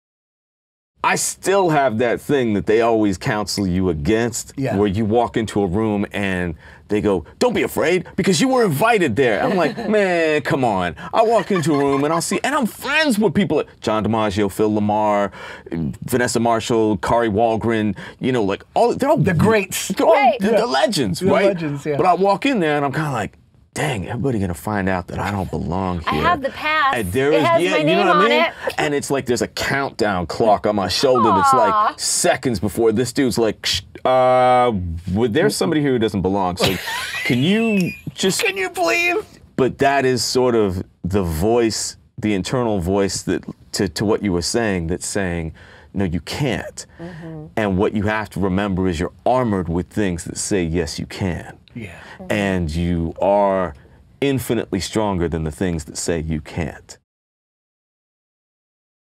Andrew Kishino - You Are Infinitely Stronger Than The Word CAN'T
VO Buzz Weekly Quick Tips. Voice over industry pros share tips and advice to help you grow your voice over business. This VO Buzz Weekly Quick Tip Video features Los Angeles animation and video game voiceover actor, Andrew Kishino.